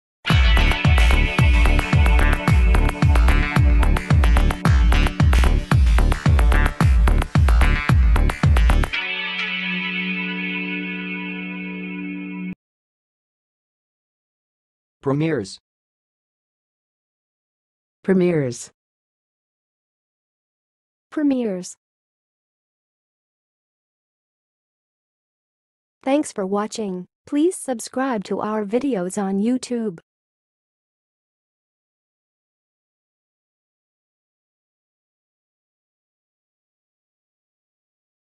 How to say "première's"! (High Quality Voices)
Watch in this video how to say and pronounce "première's"!